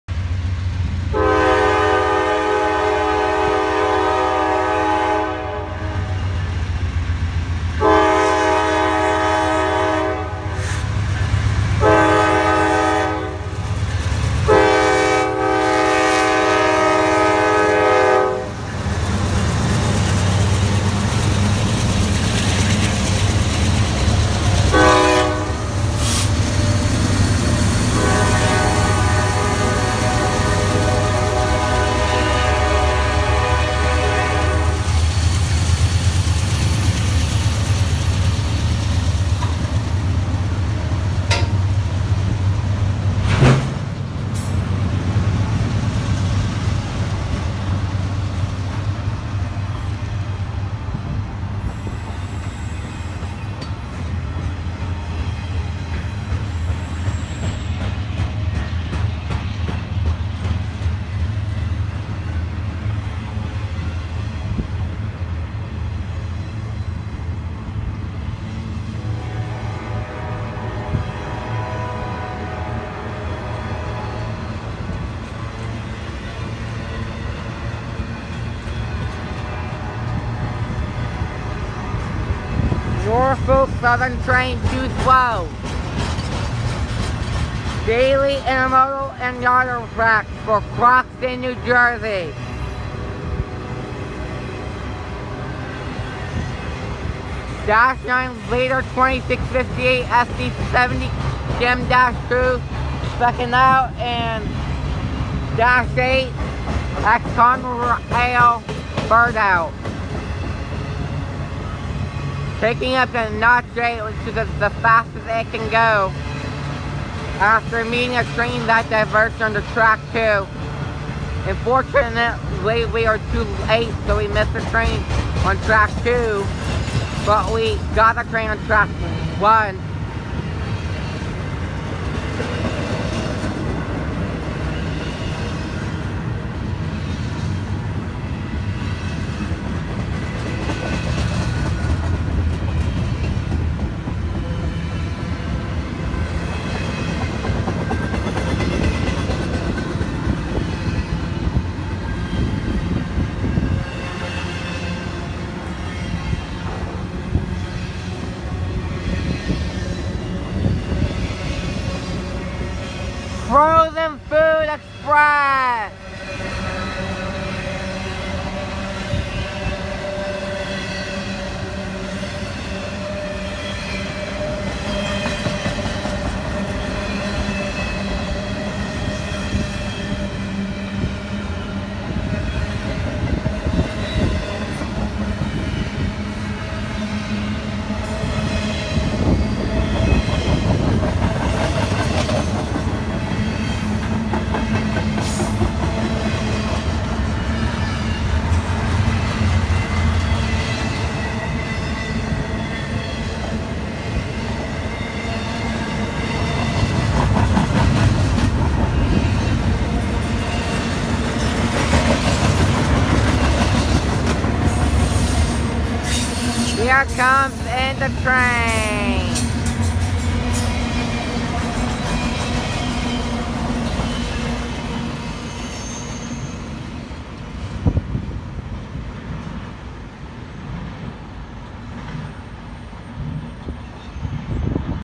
NS 212 With a Dash 9 Leader Notches up to Track Speed after Being Stopped to Meet NS 211 @ CP Duluth
NS 212 With a Dash 9 Leader Notches up to Track Speed after Being Stopped to Meet NS 211.
NS 212's Counterpart is NS 211. NS 212 Runs normally with 3 Locomotives and Runs from Atlanta,GA to Croxton,NJ . It's Next Stop for a Crew Change would be @ Greenville,SC.
NS 212's Lashup will Follow Below :
1st NS Dash 9 # 9171
2nd SD70M-2 # 2658
Lastly  NS 8930 , Not Sure of Locomotive Type
Note There are 3 Autoracks , 1 has Grand Trunk Reporting Marks and 1 has BN reporting - Last Autorack.